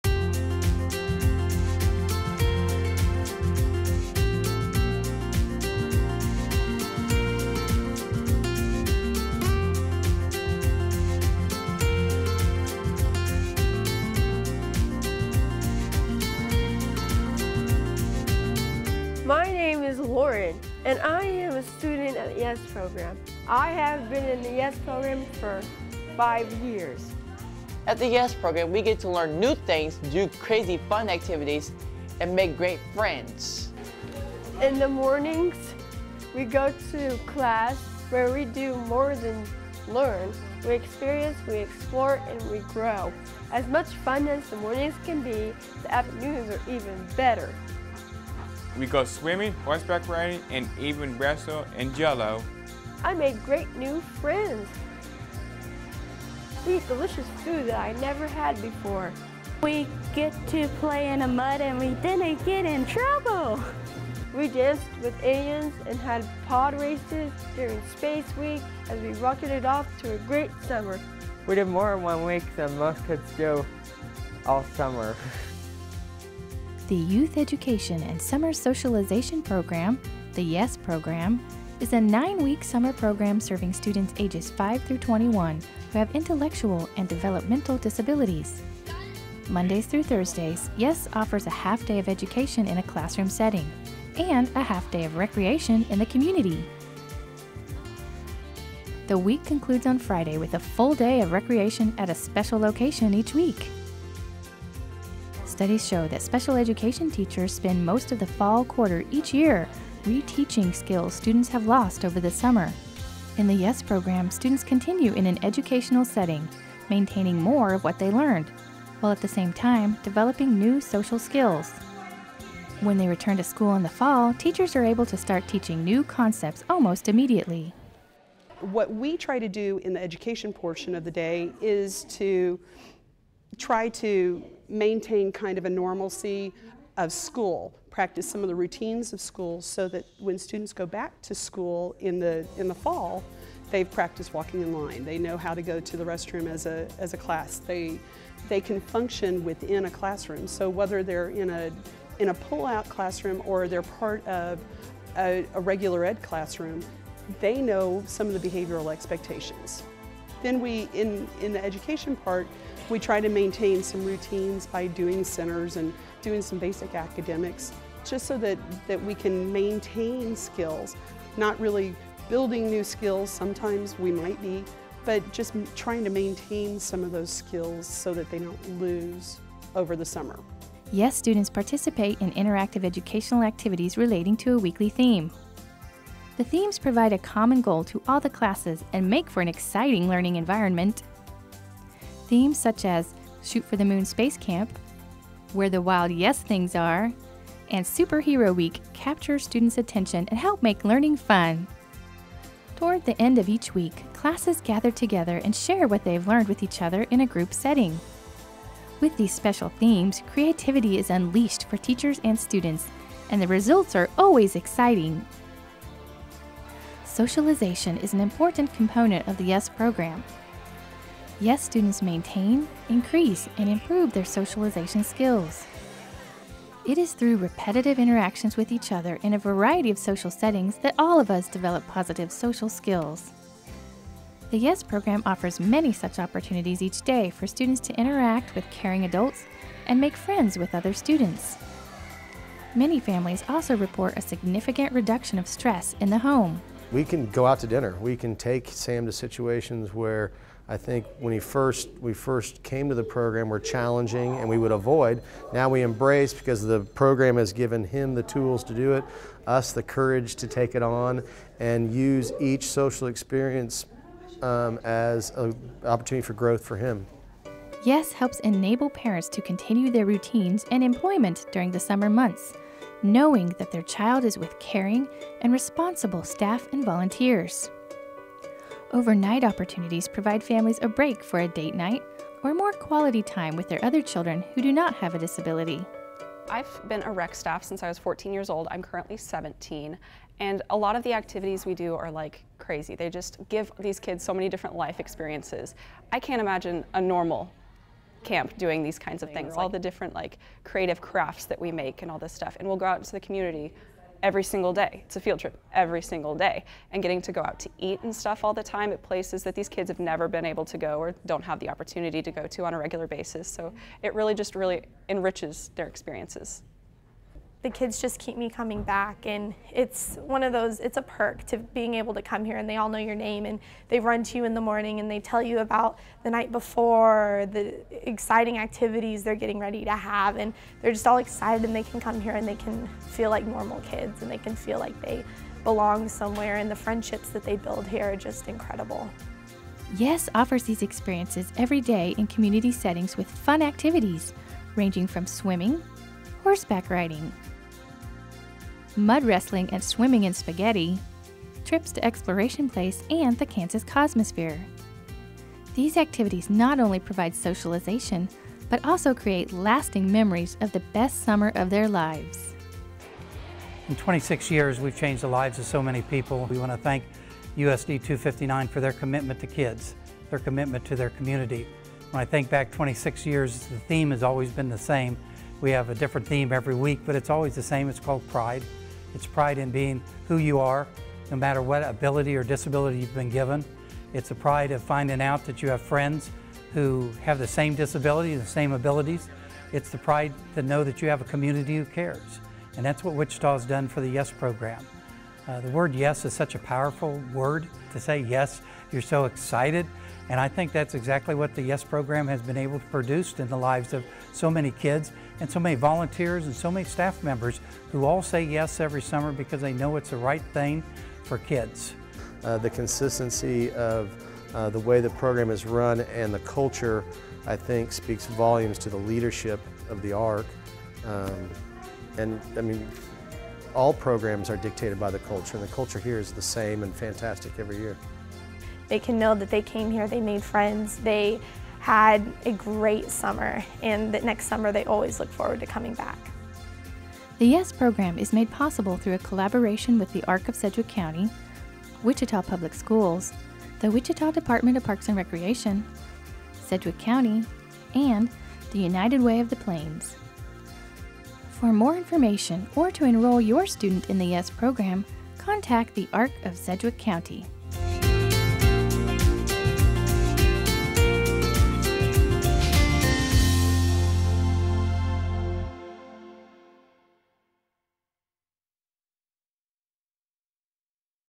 The Arc's YESS Program
YESS is an amazing summer program for students living with intellectual and or developmental disabilities.  This video overviews the fun and excitement YESS provides.the students and the benefits for them and their families.